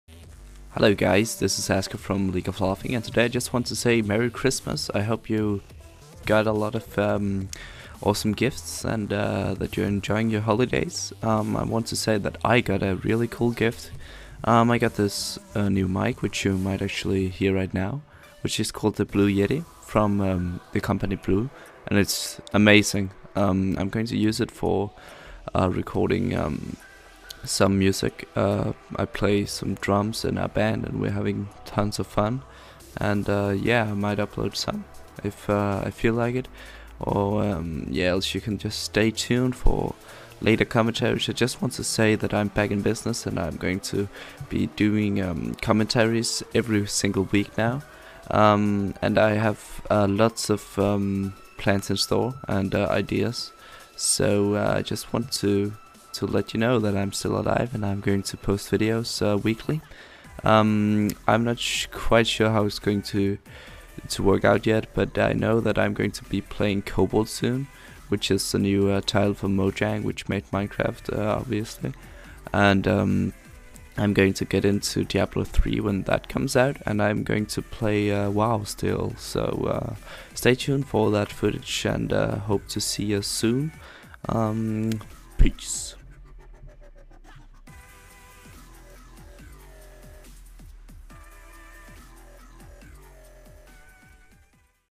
I'm back with weekly uploads
telling a bit about my plans with the channels and what games i'm going to feature... :D